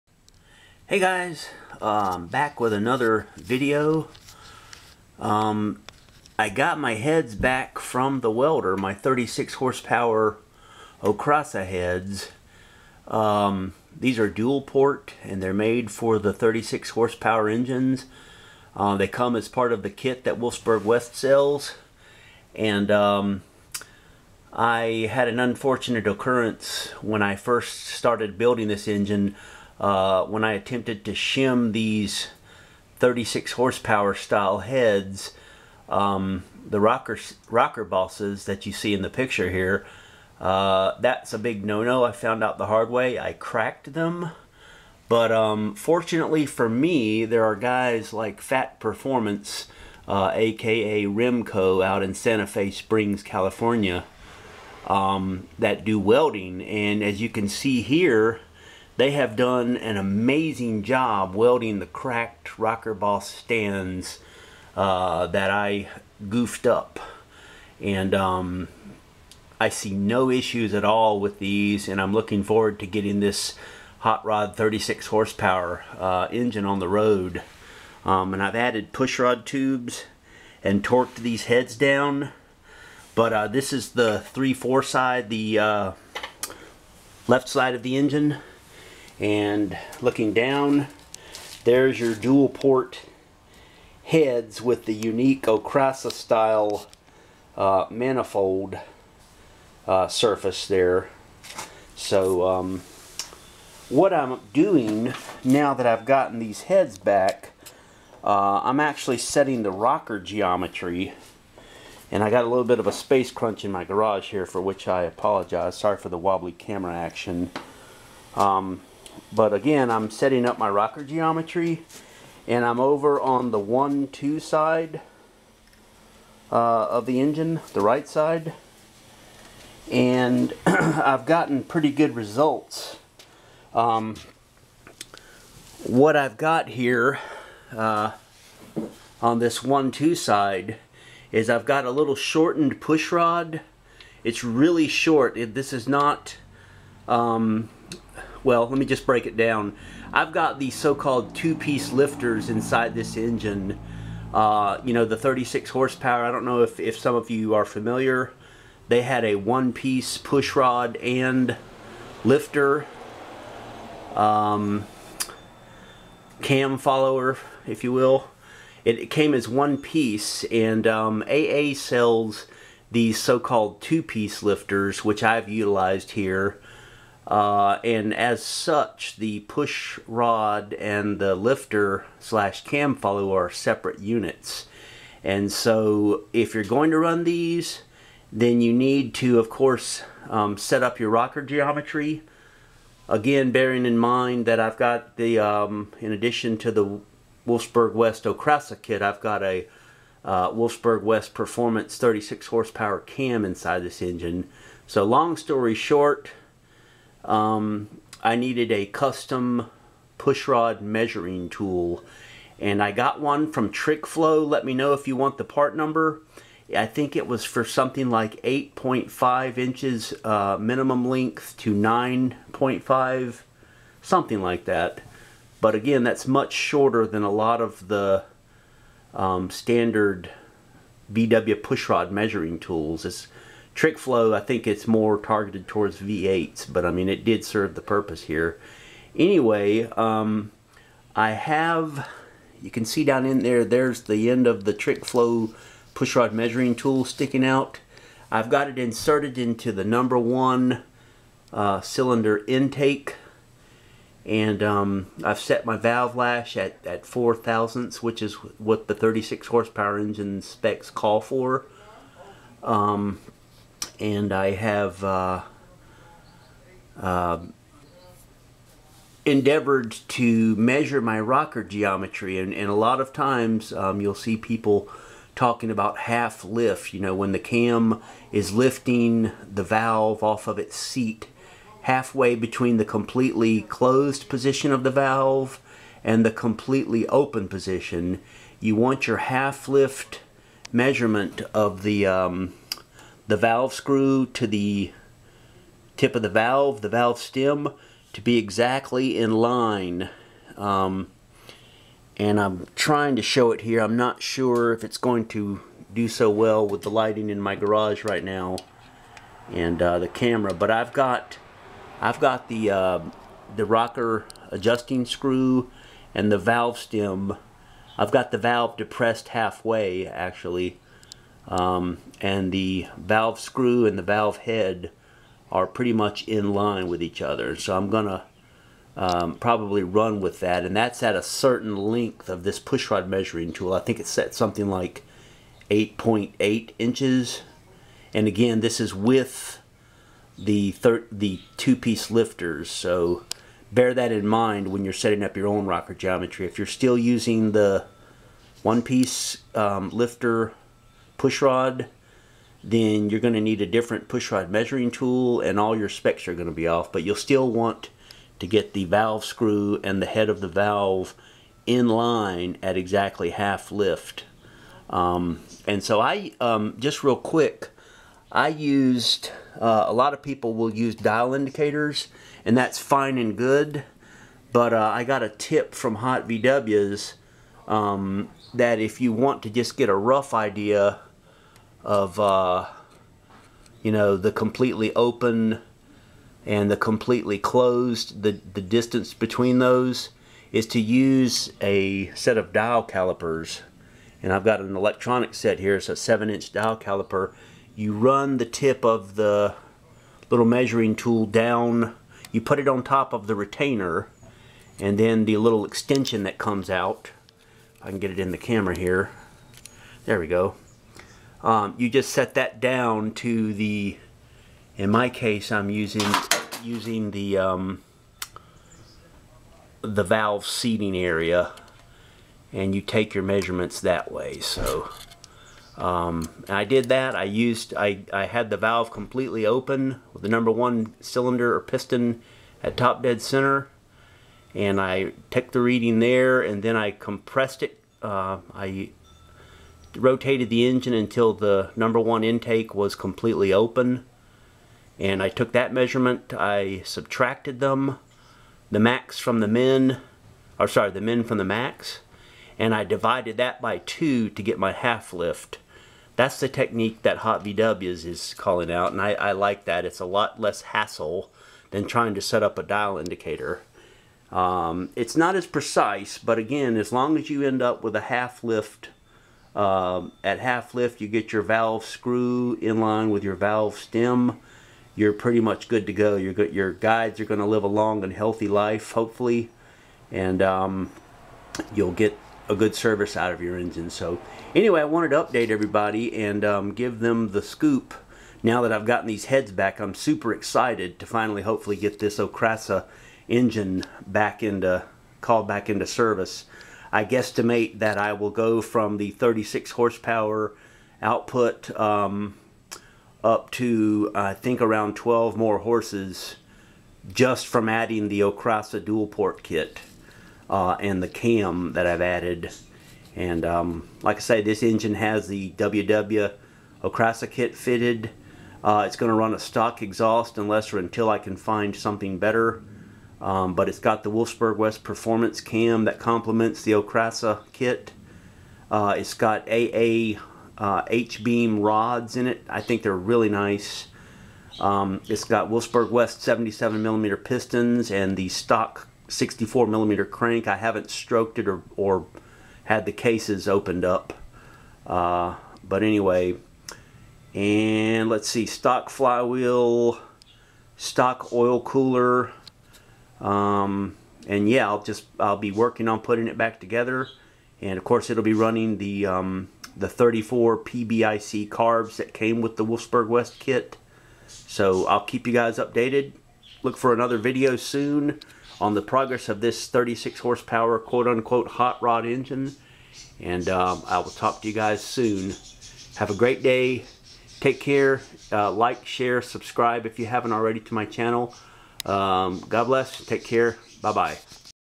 Okrasa 36hp VW Heads Back From The Welder + How To Set Rocker Geometry
I had FAT Performance/RIMCO out in CA weld the cracked Wolfsburg West Okrasa heads in my 36hp hot rod kit, and they look great! Also I present an alternate means of checking and setting rocker geometry. I hope you find this video helpful and informative!